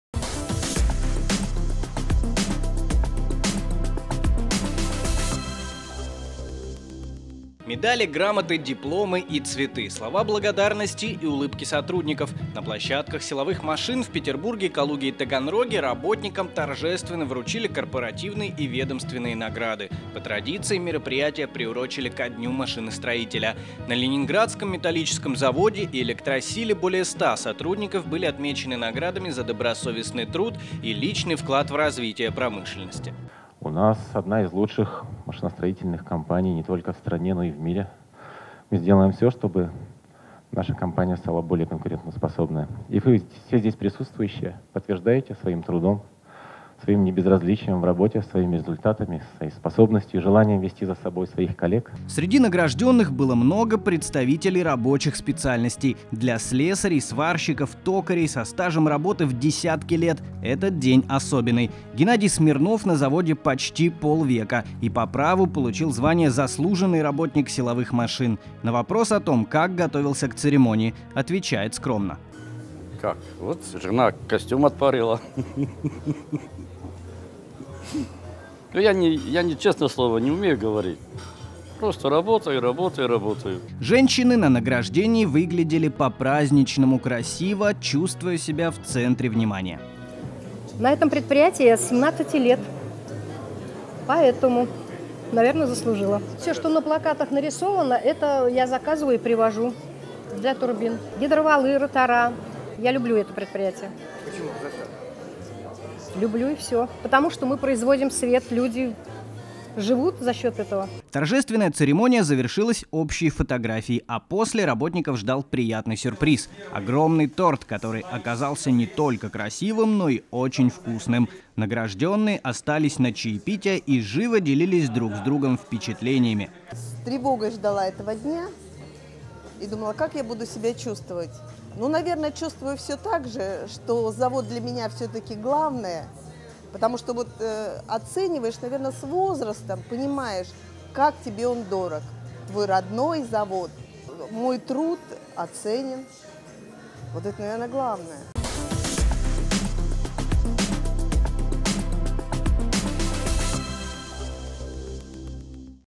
Награждение сотрудников
На площадках «Силовых машин» в Петербурге, Калуге и Таганроге работникам торжественно вручили корпоративные и ведомственные награды. По традиции мероприятие приурочено ко Дню машиностроителя.